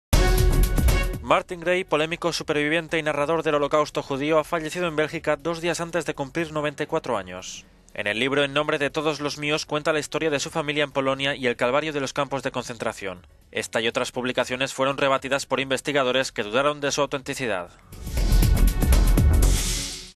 Muere Martin Gray, superviviente del Holocausto
Martin Gray, polémico superviviente y narrador del Holocausto judío, ha fallecido en Bélgica dos días antes de cumplir 94 años.